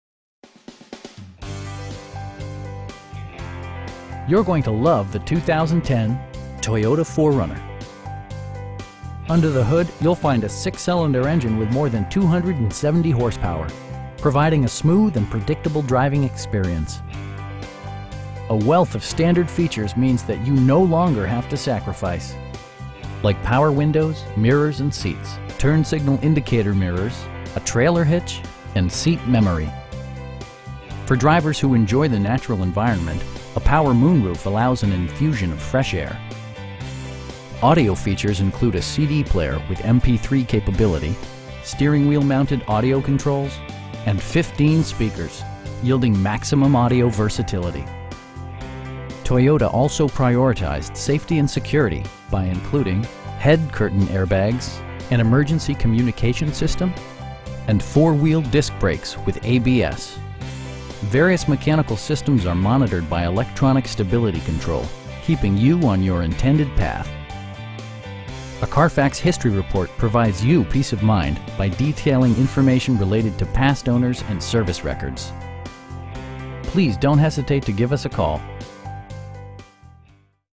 2010 Toyota 4Runner Limited V6 in Lubbock, TX 79412
You're going to love the 2010 Toyota 4Runner. Under the hood you'll find a 6 cylinder engine with more than 270 horsepower, providing a smooth and predictable driving experience. Four wheel drive allows you to go places you've only imagined. A wealth of standard features means that you no longer have to sacrifice. Like power windows, mirrors, and seats, power trunk closing assist, a built-in garage door transmitter, a trip computer, turn signal indicator mirrors, an overhead console, a trailer hitch, and seat memory. Features such as automatic climate control and leather upholstery prove that economical transportation does not need to be sparsely equipped. For drivers who enjoy the natural environment, a power moon roof allows an infusion of fresh air. Audio features include a CD player with MP3 capability, steering wheel mounted audio controls, and 15 speakers, providing excellent sound throughout the cabin. Toyota also prioritized safety and security by including: head curtain airbags, front SIDE impact airbags, traction control, brake assist, anti-whiplash front head restraints, a security system, an emergency communication system, and 4 wheel disc brakes with ABS. Various mechanical systems are monitored by electronic stability control, keeping you on your intended path. A Carfax history report provides you peace of mind by detailing information related to past owners and service records. Our aim is to provide our customers with the best prices and service at all times. Please don't hesitate to give us a call.